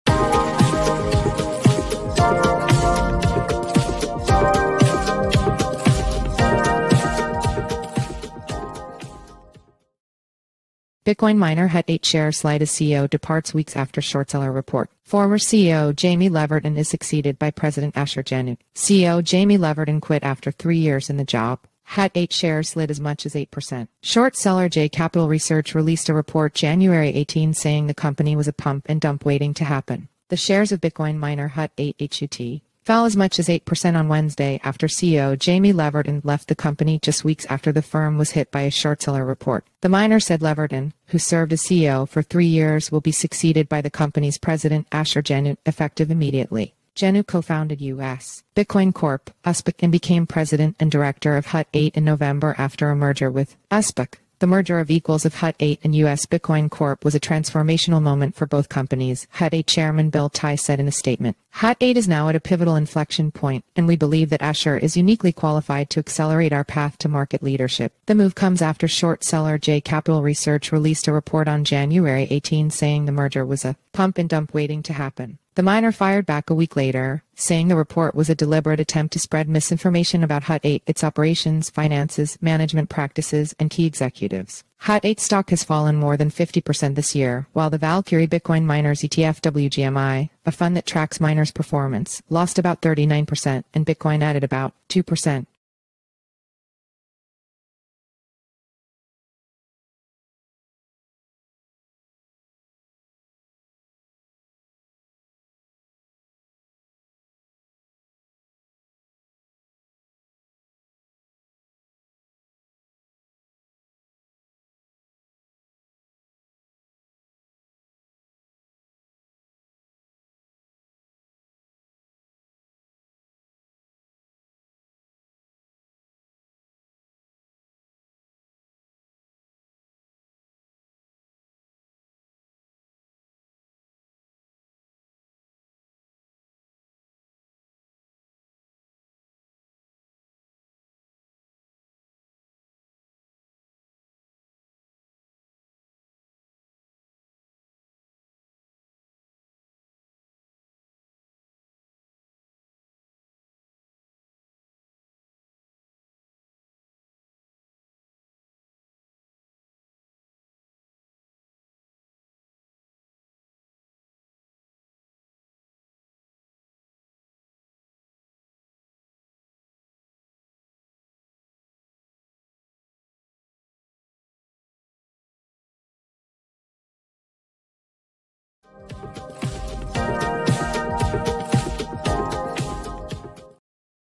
Bitcoin Miner Hut 8 Shares Slide as CEO Departs Weeks After Short-Seller Report
Former CEO Jamie Leverton is succeeded by president Asher Genoot.

CEO Jamie Leverton quit after three years in the job.

Hut 8 shares slid as much as 8%.

Short seller JCapital Research released a report Jan. 18 saying the company was a "pump and dump" waiting to happen.

The shares of bitcoin miner Hut 8 (HUT) fell as much as 8% on Wednesday, after CEO Jamie Leverton left the company just weeks after the firm was hit by a short-seller report.

The miner said Leverton, who served as CEO for three years, will be succeeded by the company's president, Asher Genoot, effective immediately. Genoot co-founded U.S. Bitcoin Corp. (USBTC) and became president and director of Hut 8 in November after a merger with USBTC.

“The merger of equals of Hut 8 and US Bitcoin Corp was a transformational moment for both companies,” Hut 8 Chairman Bill Tai said in a statement. “Hut 8 is now at a pivotal inflection point, and we believe that Asher is uniquely qualified to accelerate our path to market leadership."

The move comes after short seller JCapital Research released a report on Jan. 18 saying the merger was a "pump and dump" waiting to happen. The miner fired back a week later, saying the report was "a deliberate attempt to spread misinformation about Hut 8, its operations, finances, management practices, and key executives."

Hut 8's stock has fallen more than 50% this year, while the Valkyrie Bitcoin Miners ETF (WGMI) – a fund that tracks miners' performance – lost about 39%, and bitcoin added about 2%.